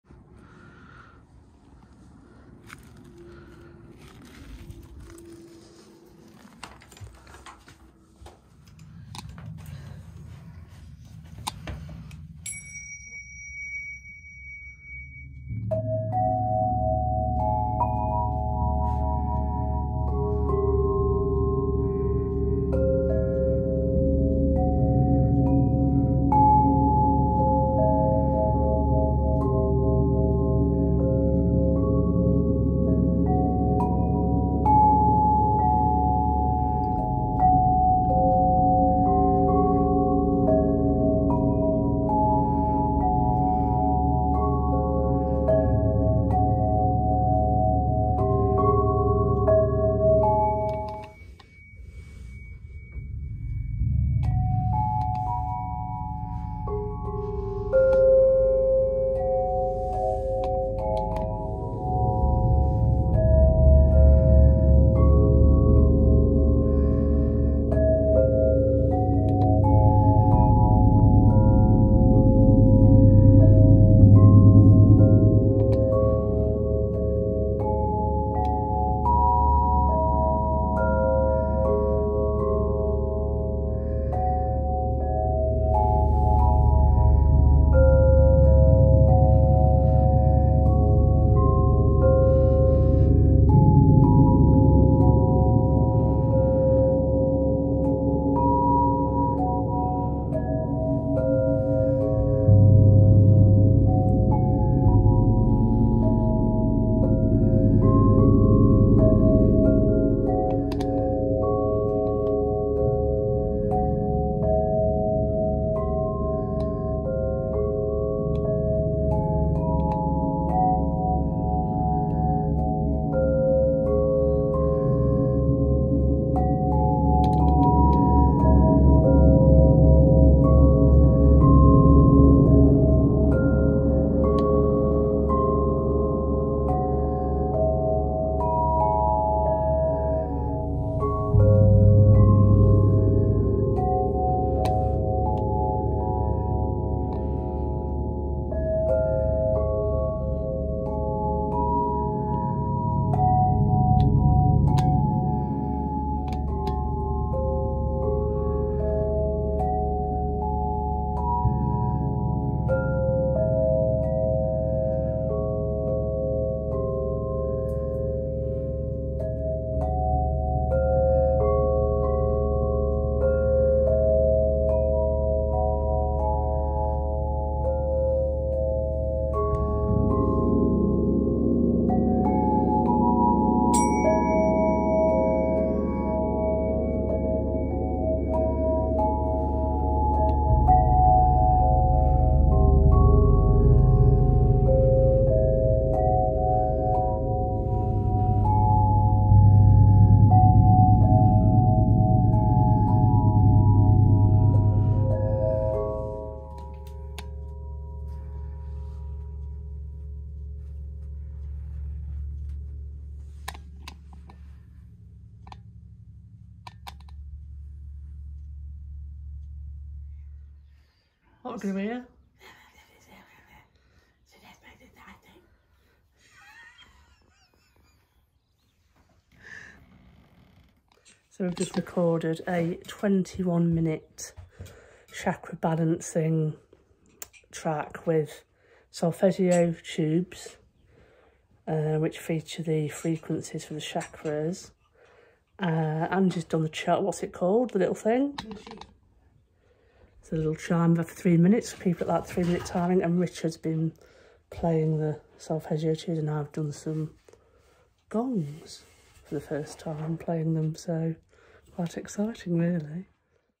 Recording Chakra Balancing with 3 minute chimes. With Solfeggio Tubes, Gongs, Tingsha Bells
Recording Chakra Balancing with 3 minute chimes. With Solfeggio Tubes, Gongs, Tingsha Bells. With Richard, Ange and Claud.

Gong therapy has been excellent for my healing journey. Especially recently. Working musically with vibrations is sublime as always.

Pyramid Sound Therapies

Fenix Flames